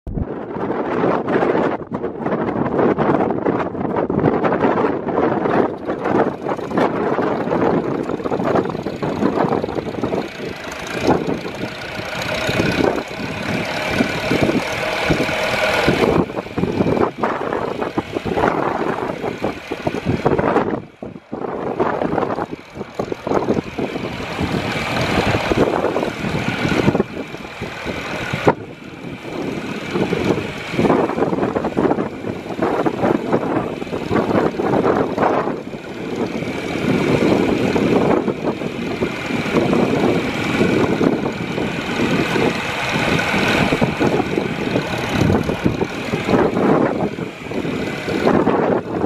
1967 JOHN DEERE 3020 For Sale
1967 John Deere 3020 2WD,
S# 112016R,
Showing 7834 Hrs,
4 Cylinder Gas,
Powershift,
Wide Front,
2 Remotes,
3Pt,
Dual PTO,
Drawbar,
12 Volt,
Fenders,
Firestone 15.5-38 Rears (NEW),
Firestone 7.5L-15 Fronts (NEW),
NEW PAINT & SEAT!!